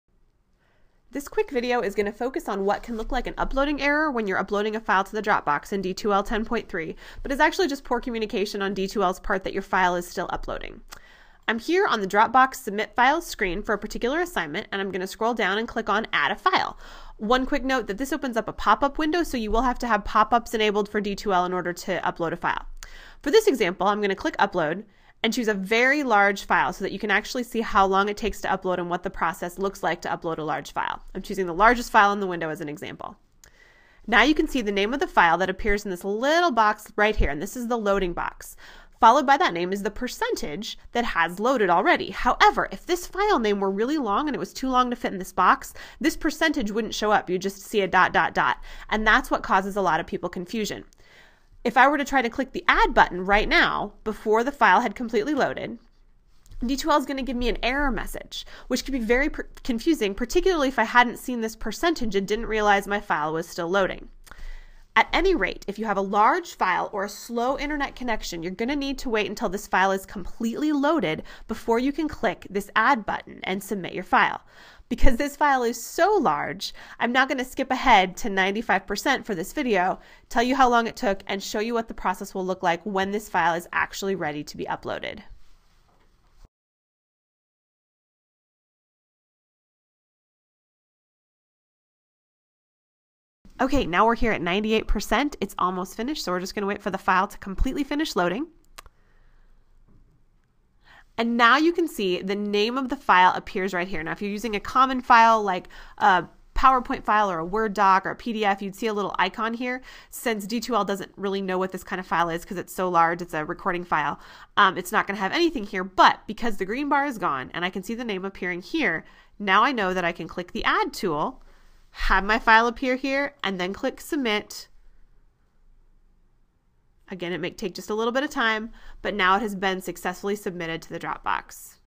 Waiting for D2L Dropbox Files to Upload
(Students and Faculty using D2L 10.3)
This video illustrates what could be misinterpreted as a loading error when students try to upload a large file or with a slow internet connection to the D2L dropbox.

Created by Instructional Technology at Oklahoma City University.